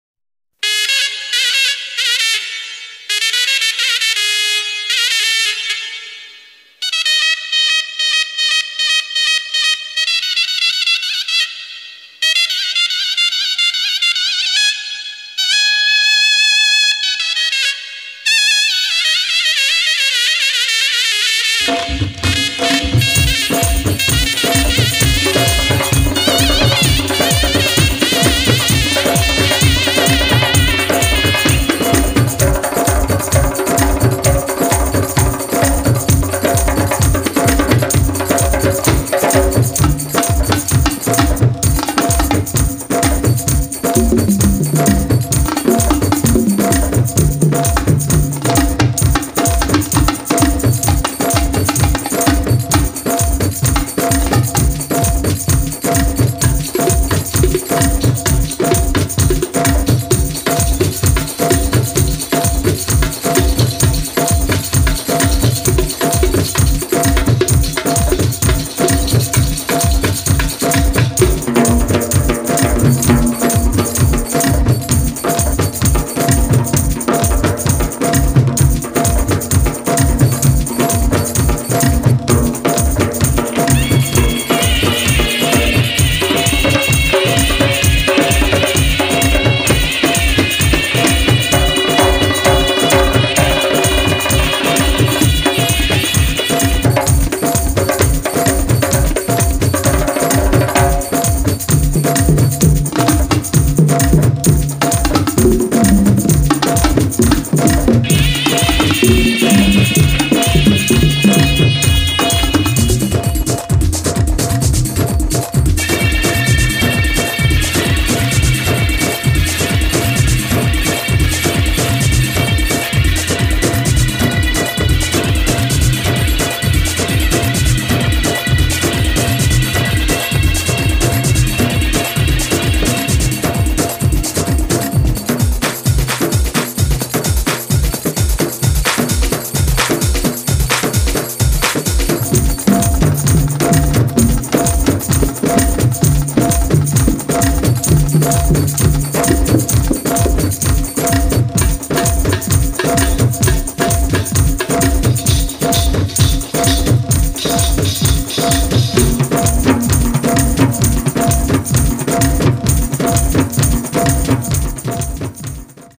Nubian music with Saidi style
This a new form of Nubian music with Saidi style (Arab Egyptian music)